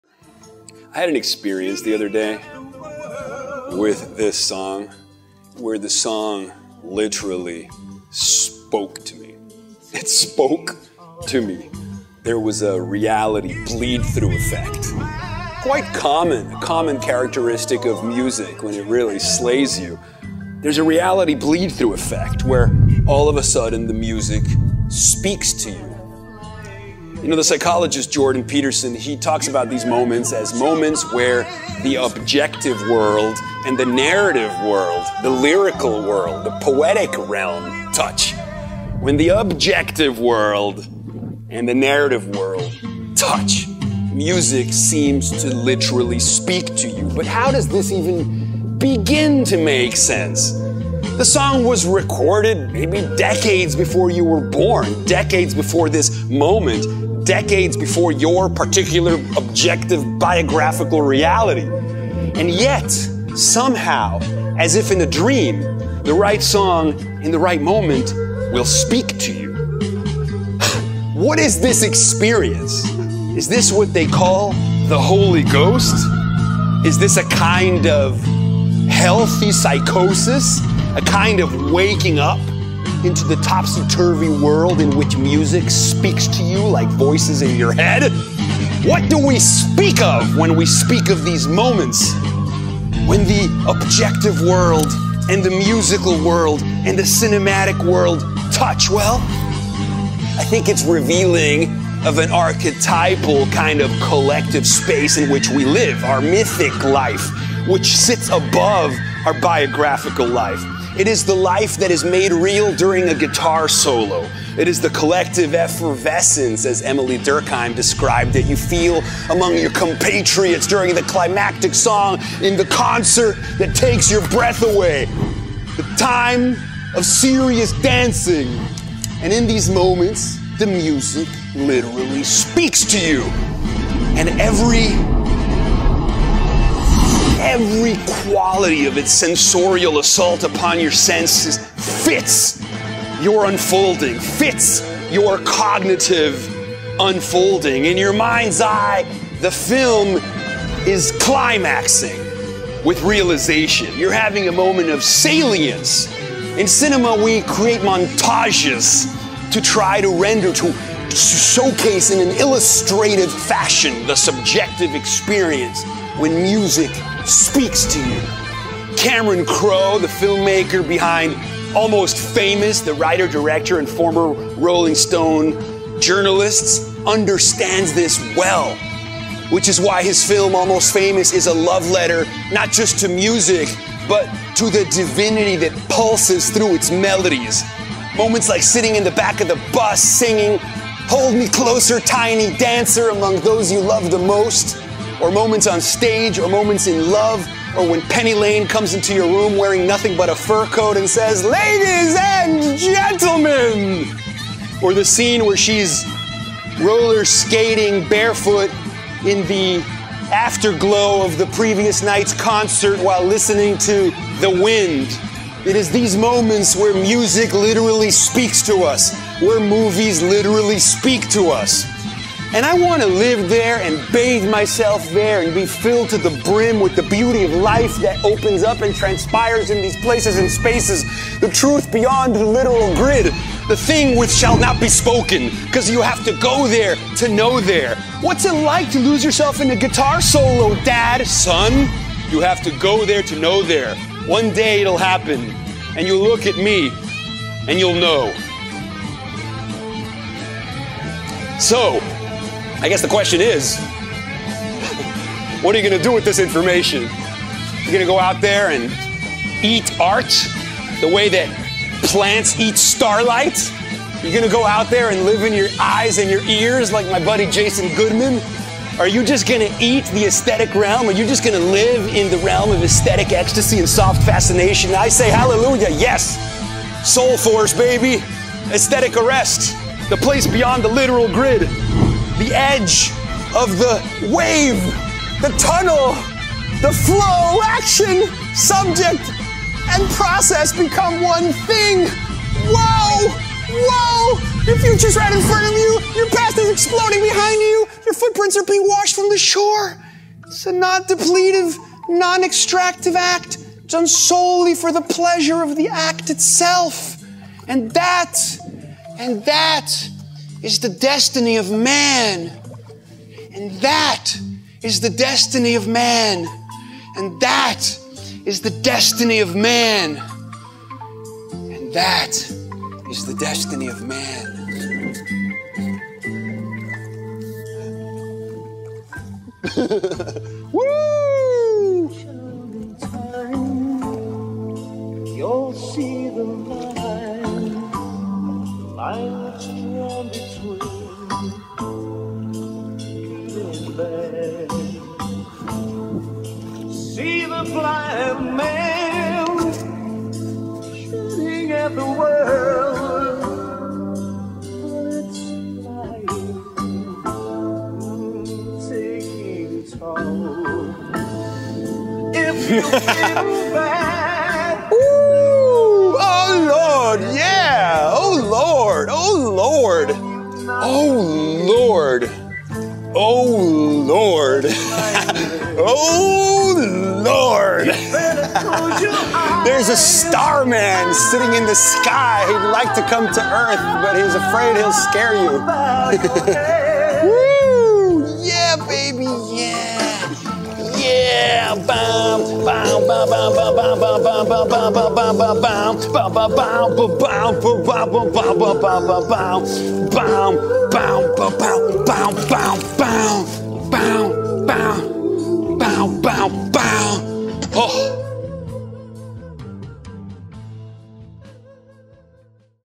HOW MUSIC SPEAKS TO YOUR SOUL
HOW MUSIC SPEAKS TO YOUR SOUL: sometimes the right song can have a reality bleed-through effect and speak directly to your deepest core. But how HOW DOES MUSIC TOUCH YOU SO DEEPLY? How does this happen? What does it feel like? How can a piece of music recorded maybe decades before, somehow be the answer to the question that drives you, or the perfect soundtrack to your life? Watch and find out. Tag your friends who need to see this 👇👇